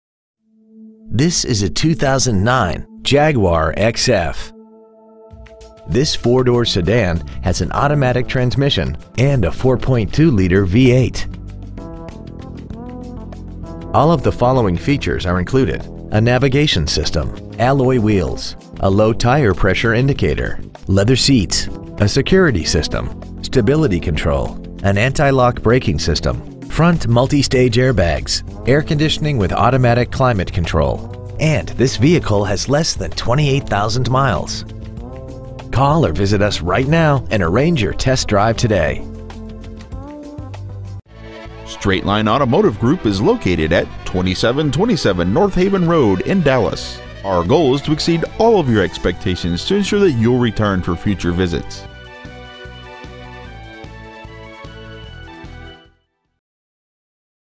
2009 Jaguar XF Dallas TX
We are proud to present this 2009 Jaguar XF .

 Year : 2009
 Make : Jaguar
 Model : XF
 Engine : 4.2L DOHC 32-valve V8 engine
 Trans . : Automatic
 Exterior : Blue
 Miles : 27,489
 Interior : Tan